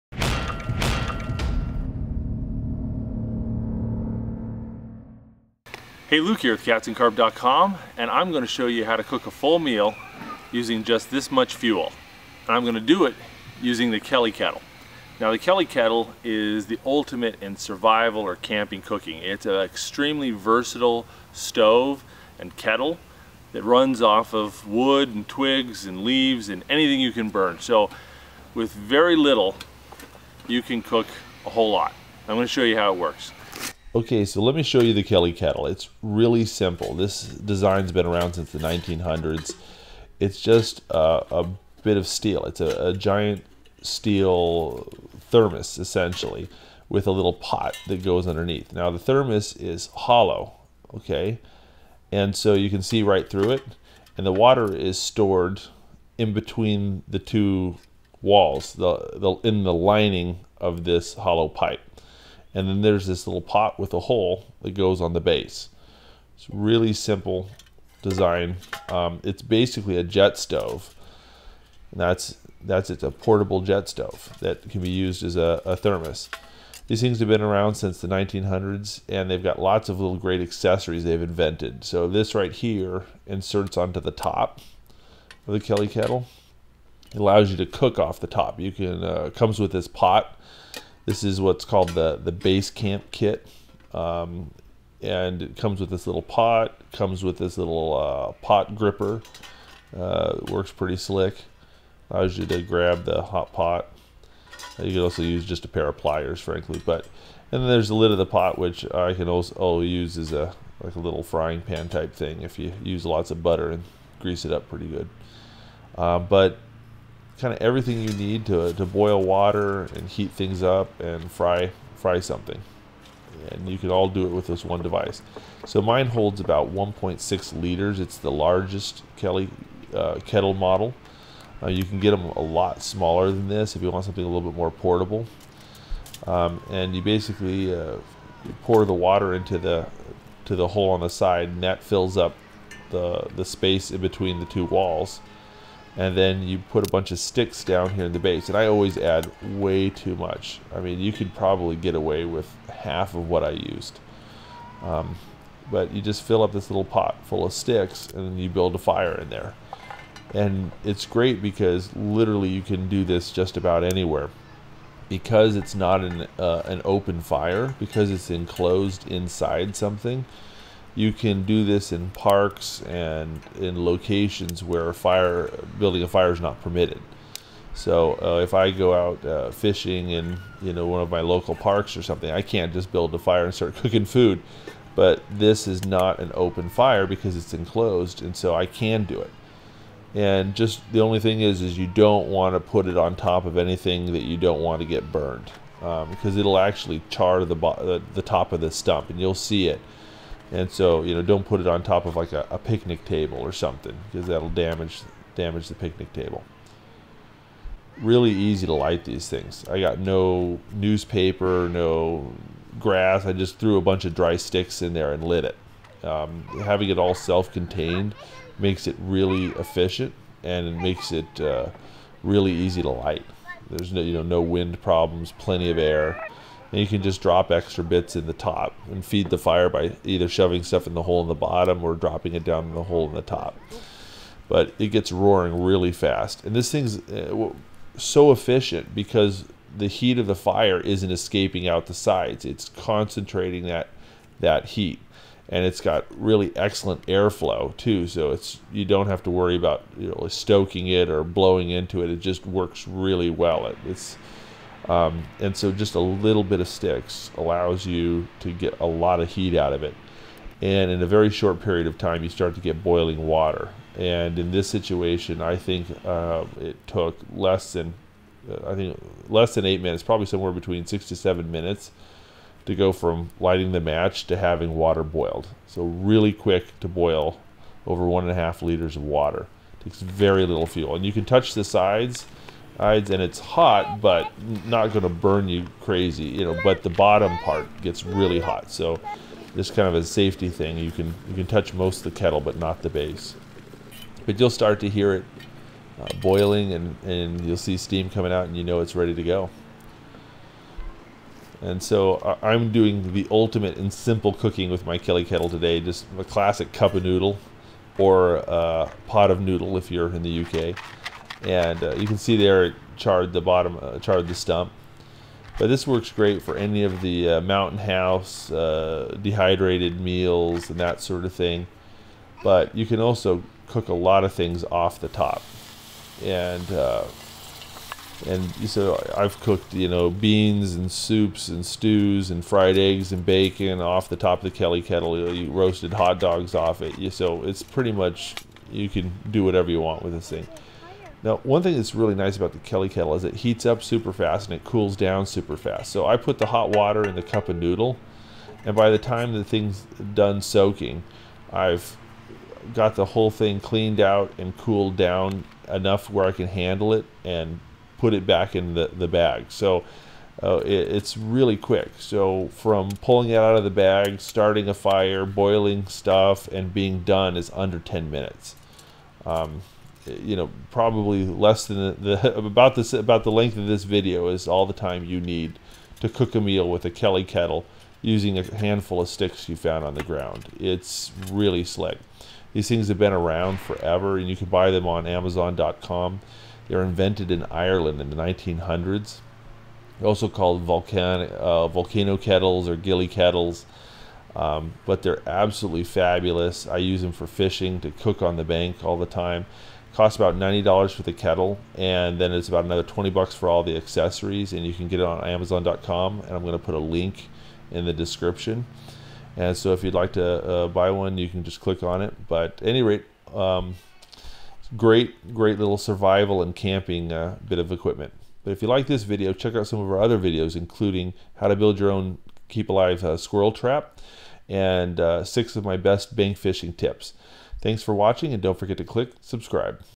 Survival cooking stove. Renewable fuel camping stove - Kelly Kettle
I love my Kelly Kettle. Unlimited renewable fuel, super versatile, and portable. I use mine all the time for camping and fishing trips but in survival situations access to unlimited renewable fuel makes it far superior to traditional stoves.
You can buy a Kelly Kettle on Amazon.com or go